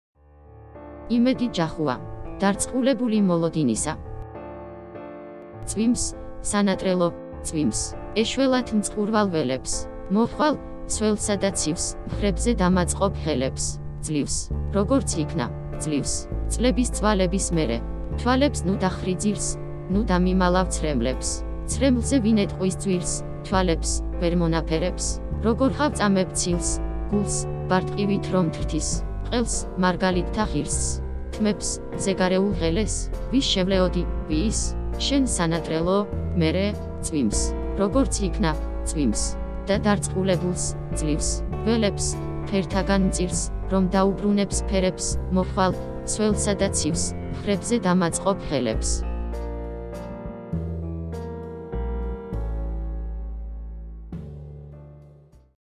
#99 იმედი ჯახუა - დარწყულებული მოლოდინისა. კითხულობს "რობოტი"
იმედი ჯახუა - დარწყულებული მოლოდინისა.
კითხულობს "რობოტი".

წვიმს,  სანატრელო,  წვიმს!..
-  ეშველათ  მწყურვალ  ველებს...
მოხვალ,  სველსა  და  ცივს,
მხრებზე  დამაწყობ  ხელებს...

ძლივს...  როგორც  იქნა...  ძლივს...
-  წლების,  წვალების  მერე...

თვალებს  ნუ  დახრი  ძირს  -
ნუ  დამიმალავ  ცრემლებს...
-  ცრემლზე  ვინ  ეტყვის  ძვირს
თვალებს,  ვერმონაფერებს?..

როგორღა  ვწამებ  ცილს
გულს  -  ბარტყივით  რომ  თრთის...
ყელს  -  მარგალიტთა  ღირსს...
თმებს  -  მზეგარეულ  ღელეს?!.

-  ვის  შევლეოდი,  ვის?..
-  შენ,  სანატრელო?..  მერე?!.

წვიმს...  როგორც  იქნა,  წვიმს...
და  დარწყულებულს  (ძლივს!..)
ველებს  -  ფერთაგან  მწირს,
რომ  დაუბრუნებს  ფერებს,
მხრებზე  დამაწყობ  ხელებს...